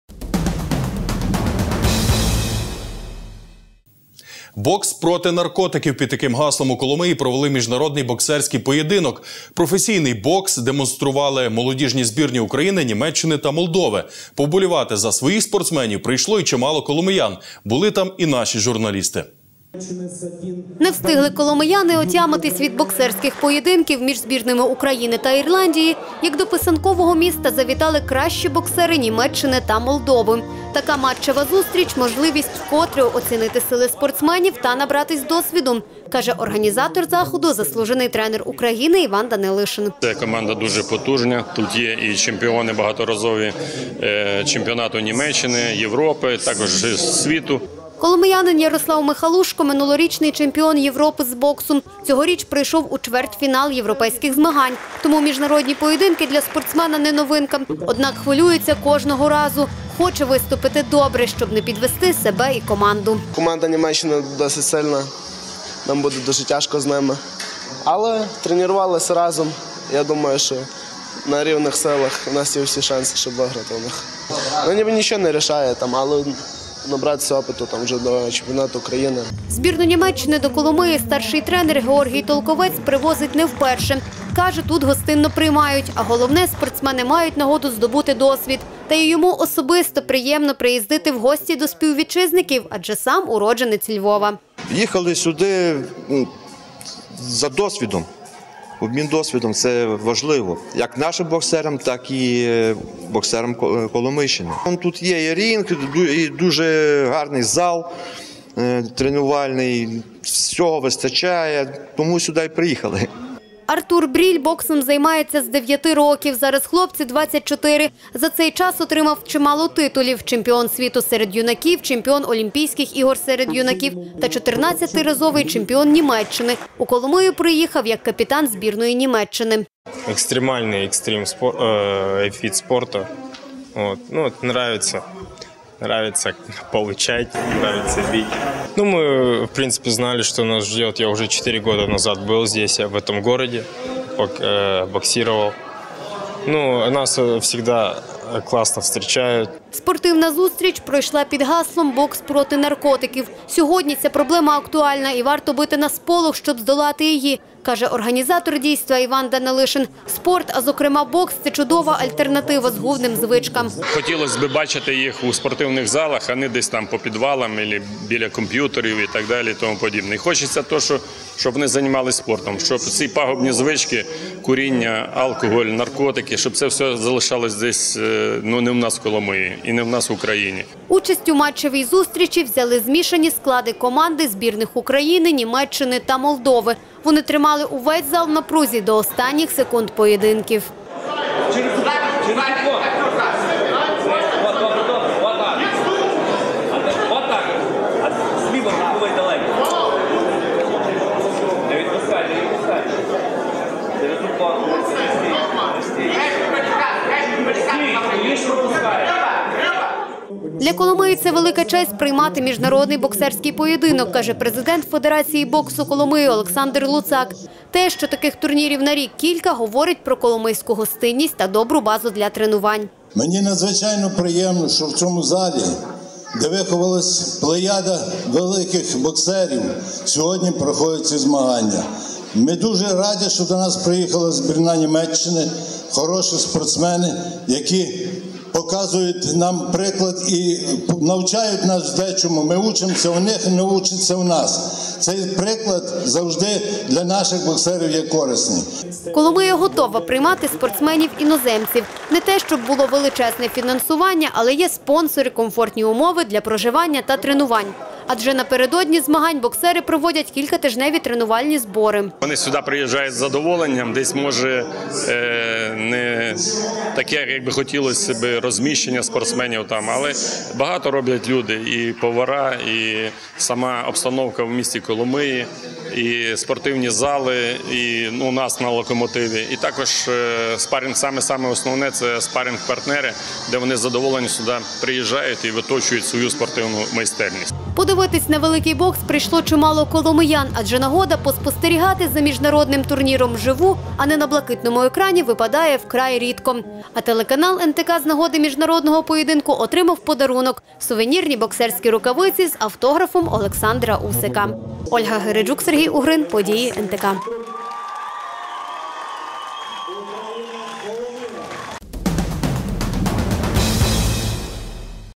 У Коломиї змагалися боксери з Німеччини
Бокс проти наркотиків. Під таким гаслом у Коломиї провели міжнародний боксерський поєдинок. Професійний бокс демонстрували молодіжні збірні України, Німеччини та Молдови. Вболівати за «своїх» спортсменів прийшло і чимало коломиян.